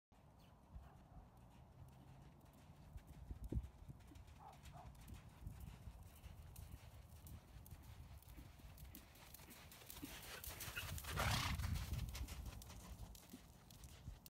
If I Can Do It, You Can Do it: Loping a Horse Bareback
Bareback riding is great for developing core strength and better balance. It also helps you get a feel for your horse. If that’s not enough, it keeps you warm and toasty in winter!

Win. Win. Win.

Tips for Riding Bareback:

Practice on a rock solid horse.
Grip with your thighs.
Move with your horse.
Don't steady yourself with the reins.
Keep your head up and look where you're riding.
Loping is easier than trotting.